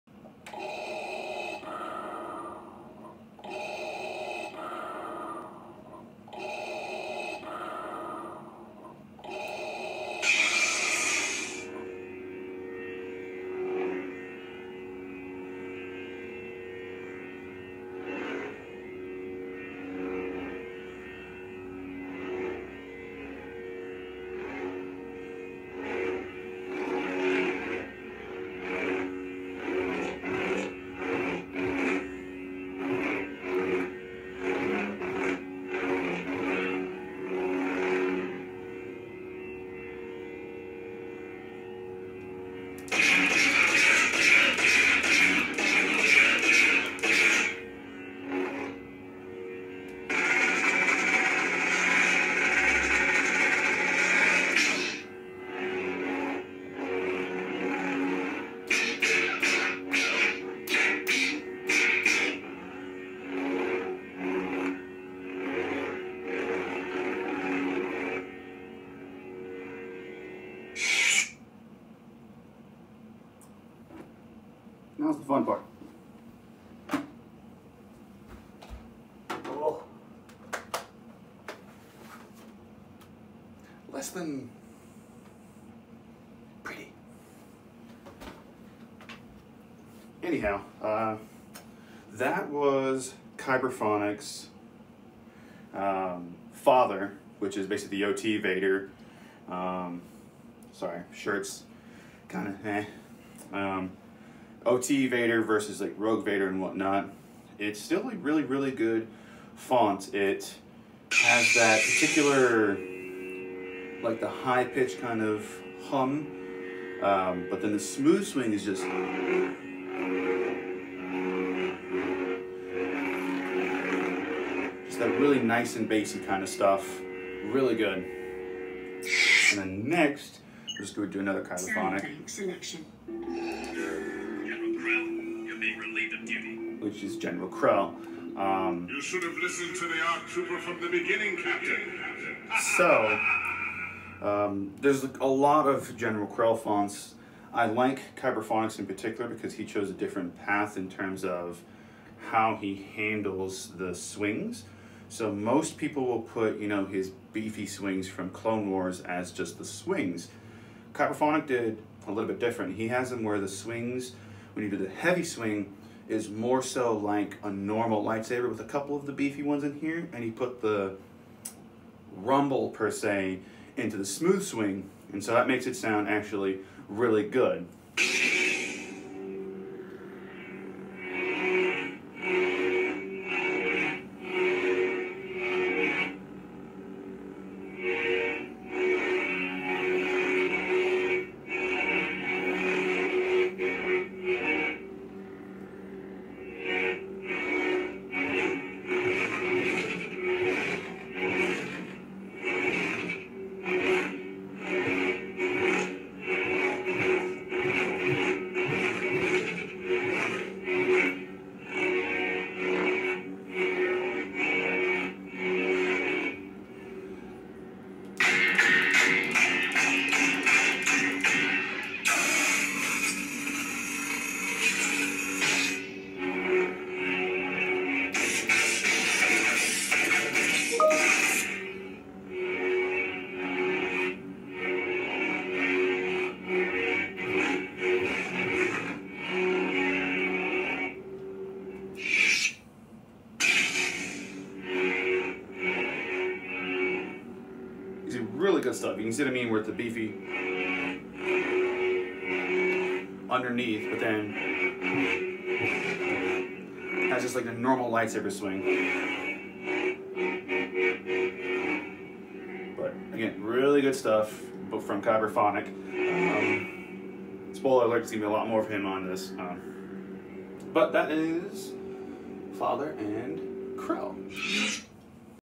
Font a day October #3 - Father and General Krell by Kyberphonic
Day number 3 and the last of the double fonts per video. Lots more to come though, enjoy!

Both fonts by Kyberphonic

Saber in video is my custom acid etched Sabertrio Reaver